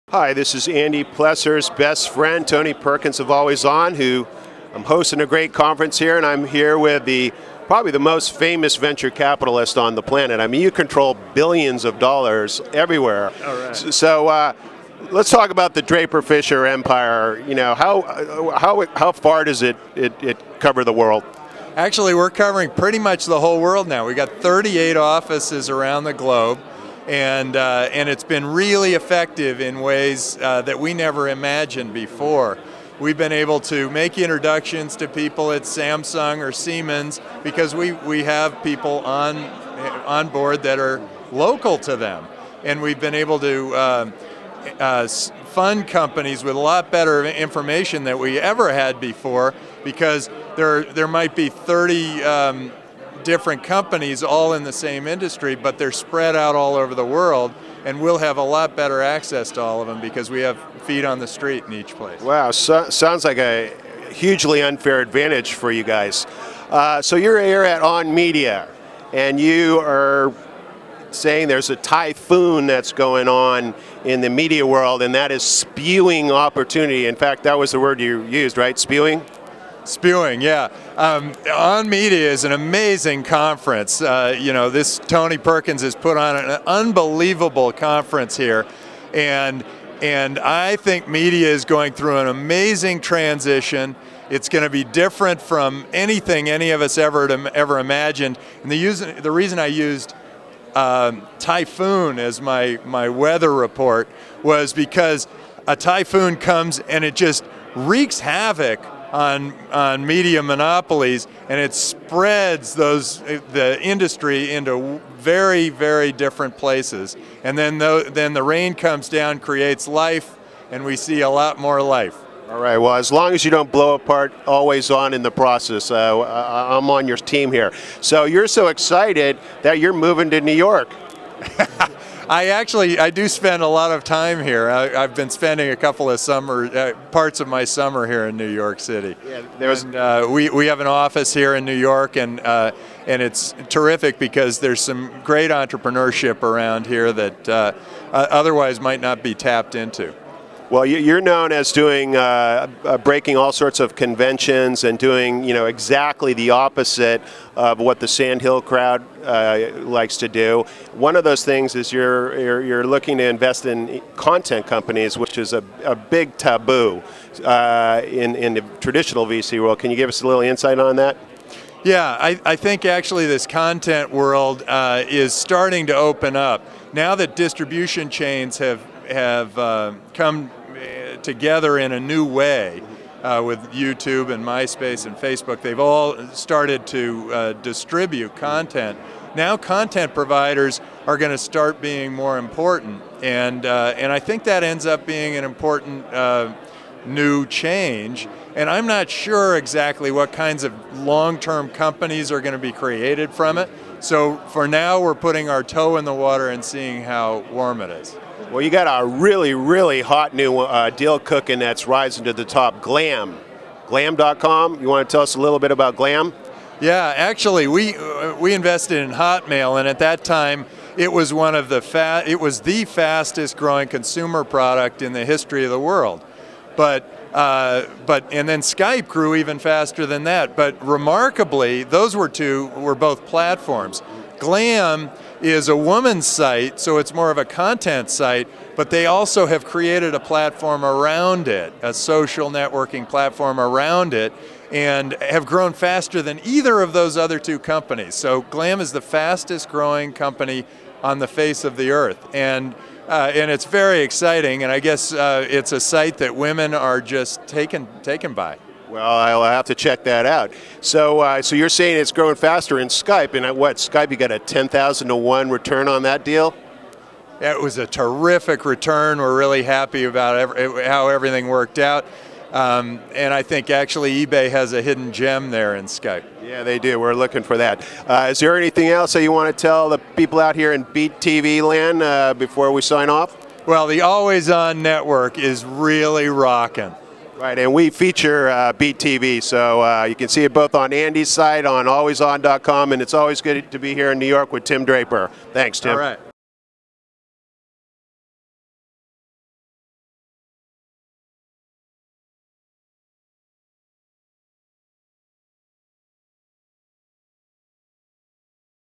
Tim Draper with Tony Perkins
VC Titan Tim Draper: "Content Providers are Becoming More Important"...And Glam is the Fastest Growing Company "On the Face of the Earth"

Tim Draper, one of the world's most successful and influential venture capitalists who manages some $7 billion in VC funds, is bullish on content companies.  This is quite a shift for the big Sand Hill Road VC's who have steered clear of content companies.

He told my buddy AlwaysOn founder Tony Perkins yesterday in this interview that content providers are becoming more important and he is starting to invest in the space.

The principal reason that content creators are becoming valuable in the eye of venture capitalists is because the avenues of online syndication have changed everything. Anyone can be a star! We agree with you Tim.

One his most high profile content company investments is Glam.com, which Draper says is the "fastest growing company on the face of  the earth."  He notes that it's growing faster than Hotmail and Skype, two of his past investments.

Tim was the keynote speaker at the Always On conference in Manhattan.  You can watch the session as a live stream today.

You can grab this video of Tony and Tim right here.

-- Andy Plesser

Posted on Wednesday, January 30, 2008